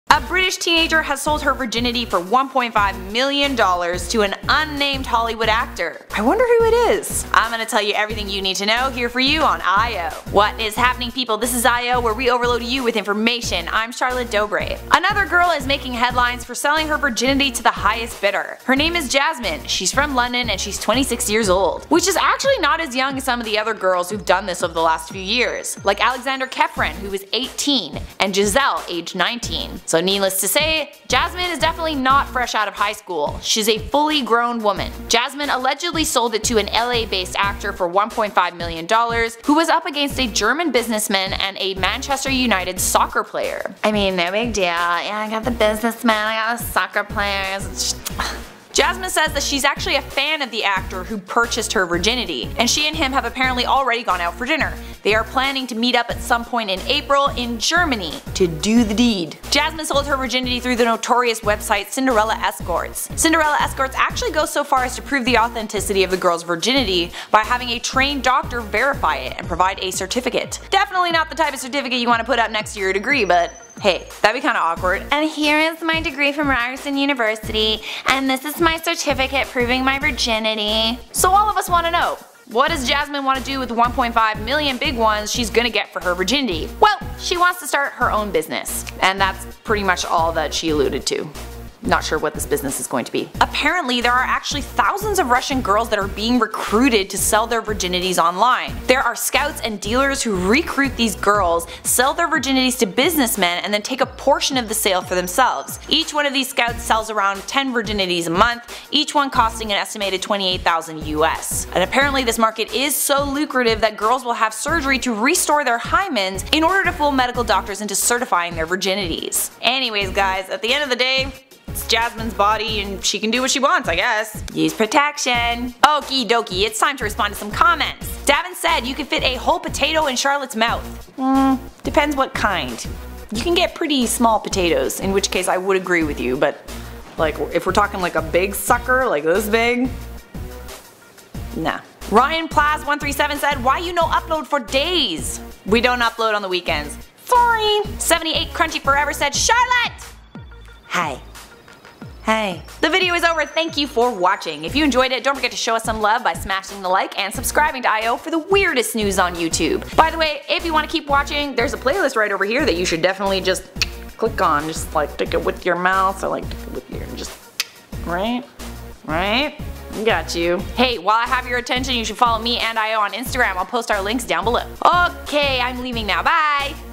Woman Sells V Card To CELEBRITY For $1.5 Million
A British woman has sold her v card for $1.5 million to a Hollywood actor but who could it be?